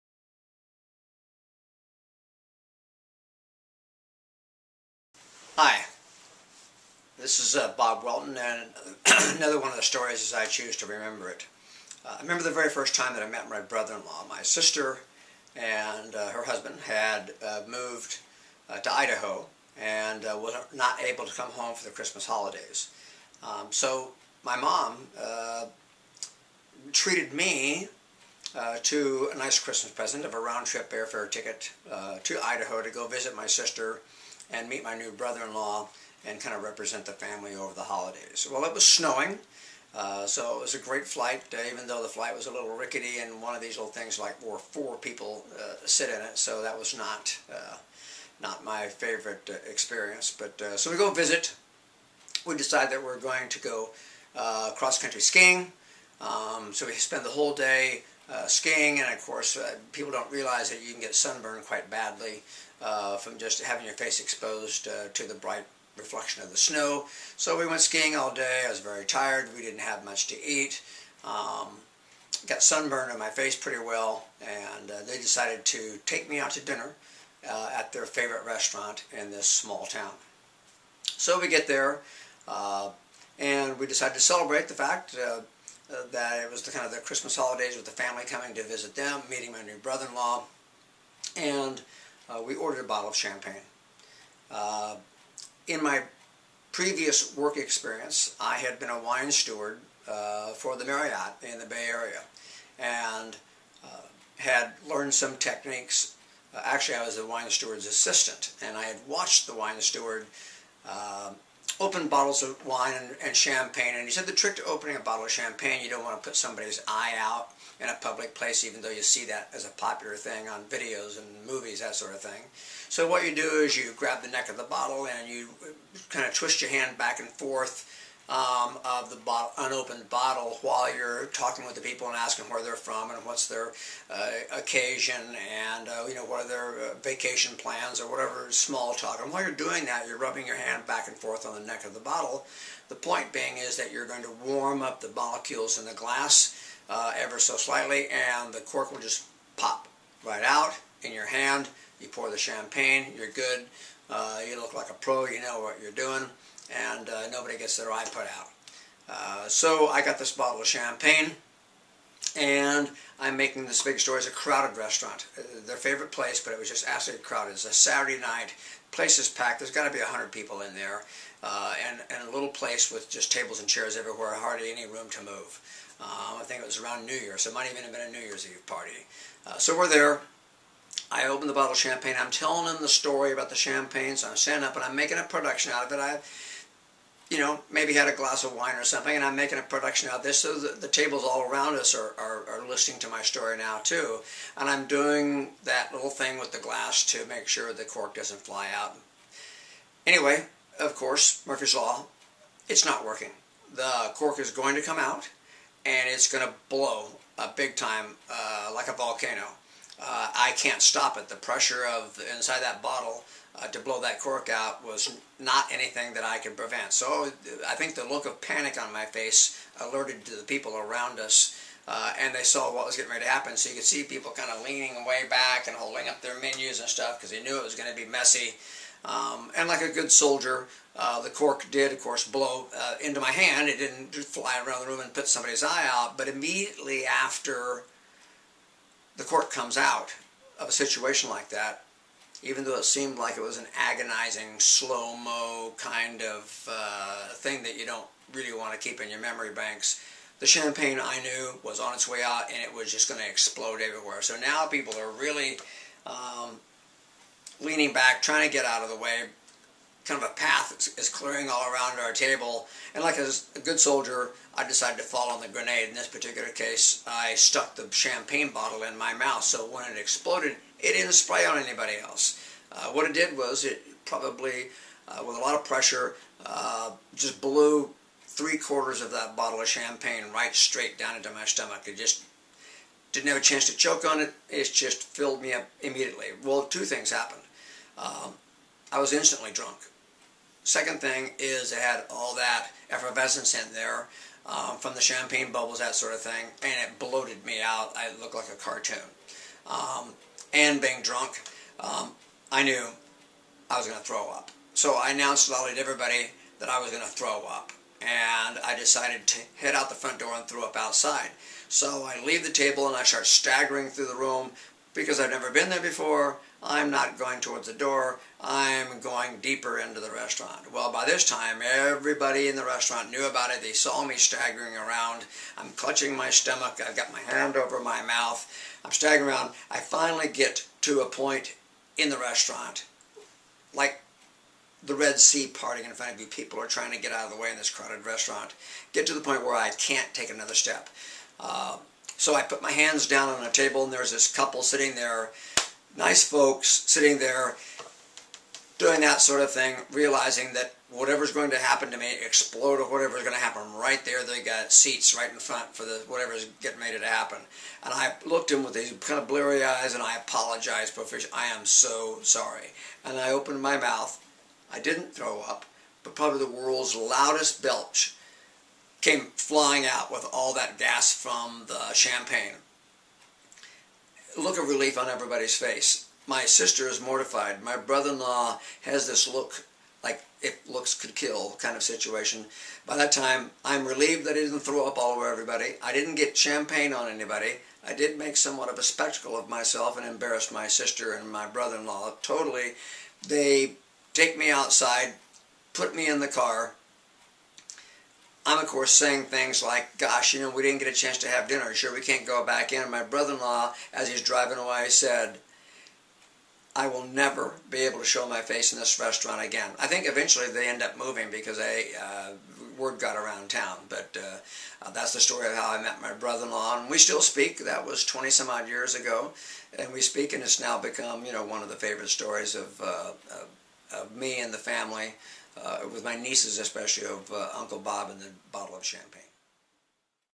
Robert Welton Remembers: The Champagne Story
What happens when you are trying to impress your new brother-n-law with opening a bottle of champagne (with flair) in a crowded restaurant and it all goes wrong.